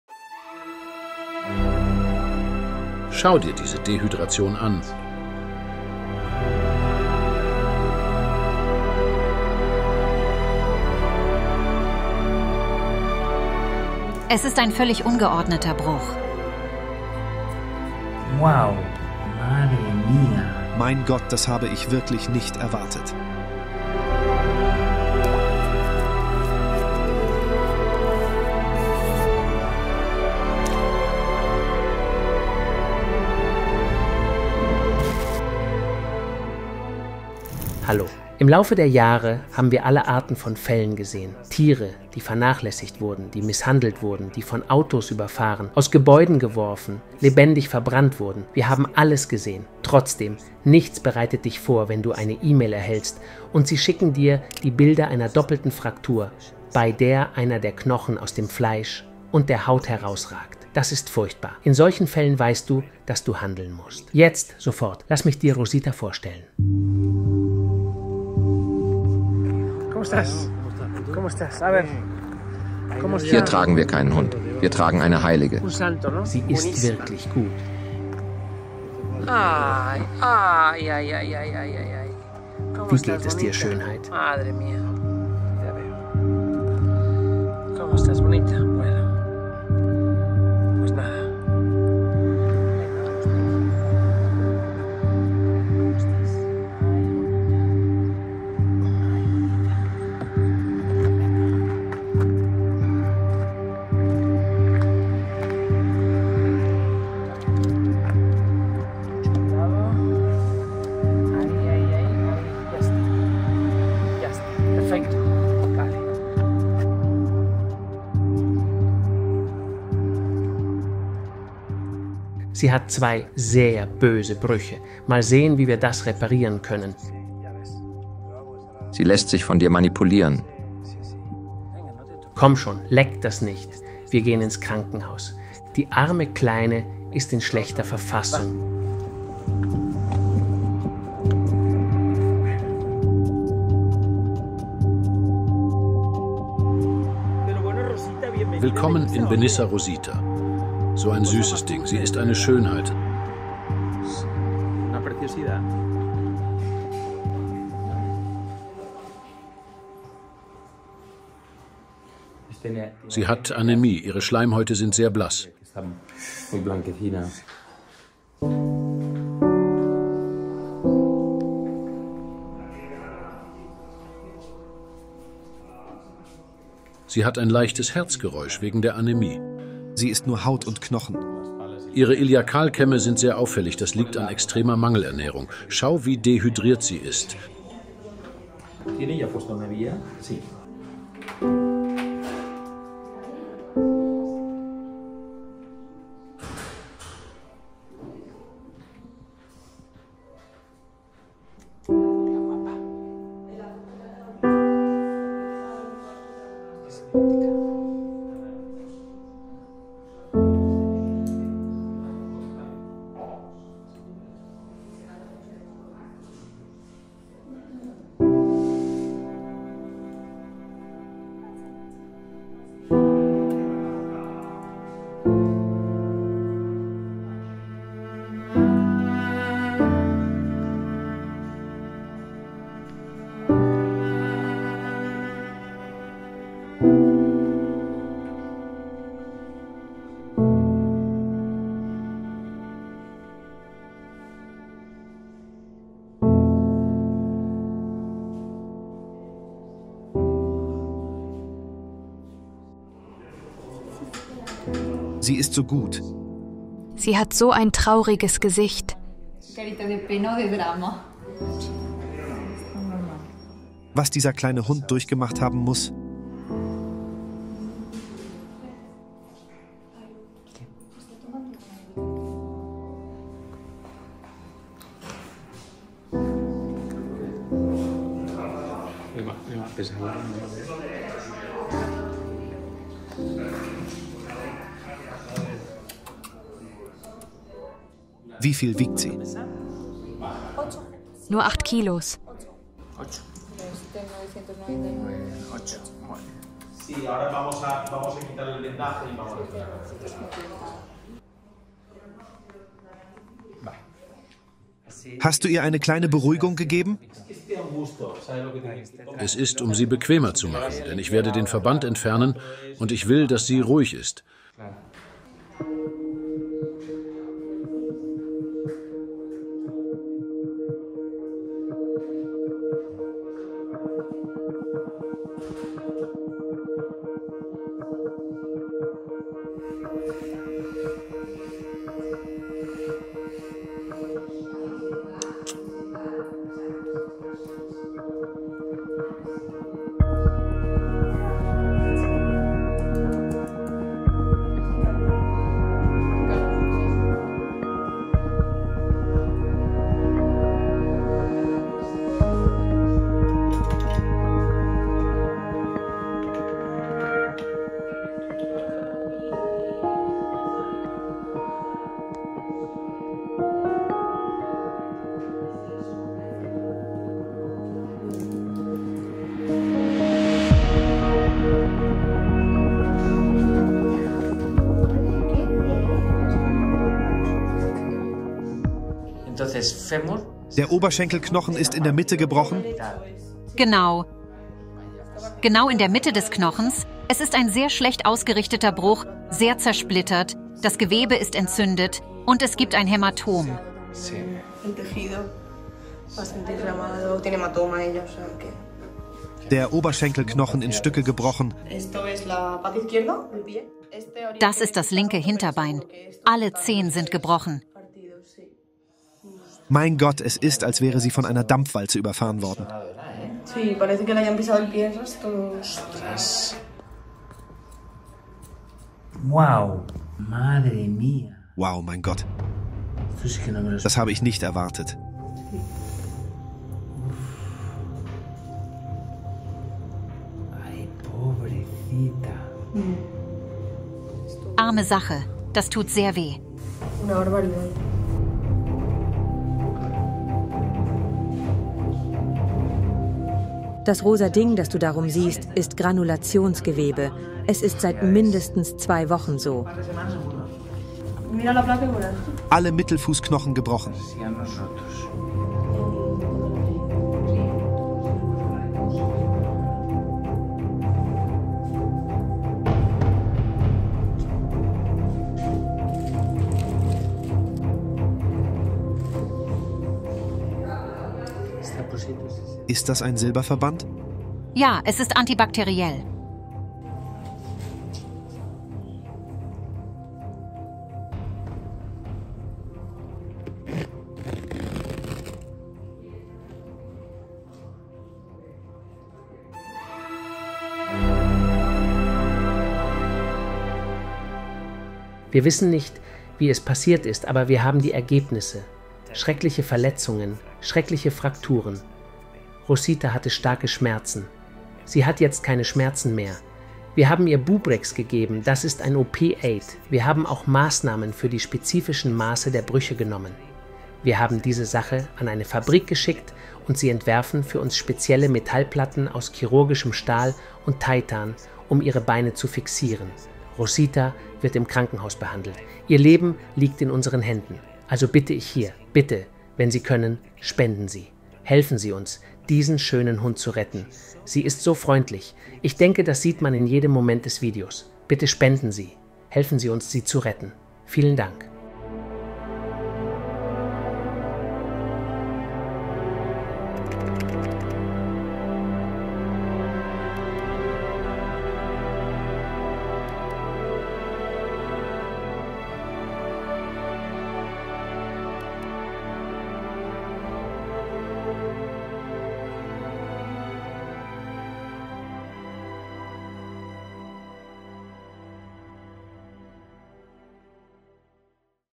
Brutaler Angriff hinterlässt Hund mit mehrfachen offenen Frakturen
Im Laufe der Jahre sind wir auf zahlreiche Fälle schwerer Misshandlung und Vernachlässigung gestoßen, wobei wir Tiere in äußerster Not gerettet haben. Doch nichts bereitet uns wirklich auf den herzzerreißenden Anblick eines freundlichen und liebevollen Hundes vor, der vor entsetzlichen Schmerzen gelähmt ist.

Heute möchte ich Ihnen von Rosita erzählen. Wir wissen nicht genau, unter welchen Umständen sie ihre Verletzungen erlitten hat, aber die Folgen sind verheerend. Rosita leidet an einem gebrochenen Oberschenkelknochen, einer beeinträchtigten Hüfte und ihr Pfötchen ist in einem besonders schrecklichen Zustand. Die Knochen ihrer Zehen sind vollständig zersplittert, was zu einer offenen Fraktur mit herausragenden Knochen führt, die durch Muskeln und Haut schneiden. Der Anblick ist unerträglich und der Schmerz, den sie ertragen musste, ist unvorstellbar.

Helft uns Rosita zu retten!